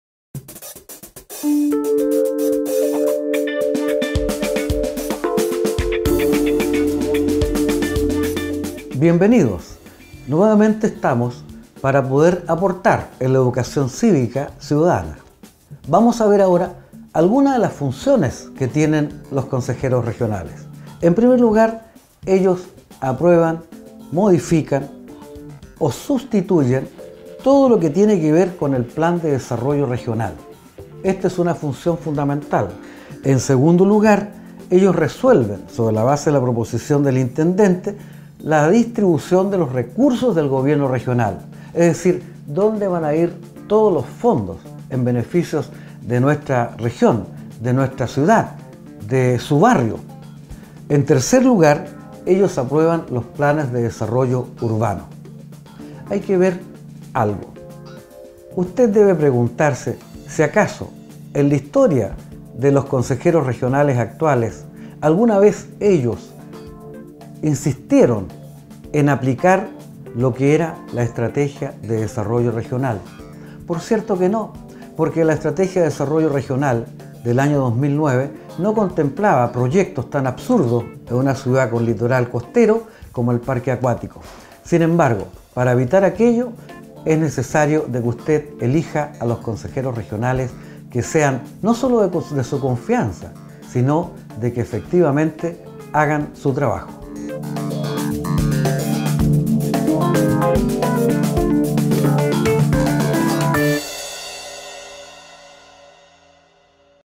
EDUCAR PARA GOBERNAR / CAPSULA 2
Próximos a la primera elección democrática de Consejeros Regionales, Hermann Mondaca Raiteri (Periodista, historiador, escritor, documentalista y ariqueño), nos da algunos elementos educativos para comprender de mejor manera cual es la labor de un Consejero Regional.